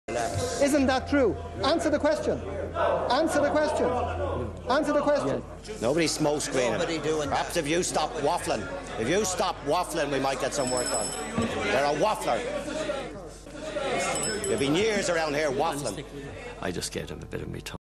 Bertie Ahern "You're a waffler" 1994
Minister for Finance Bertie Ahern taking questions from Gay Mitchell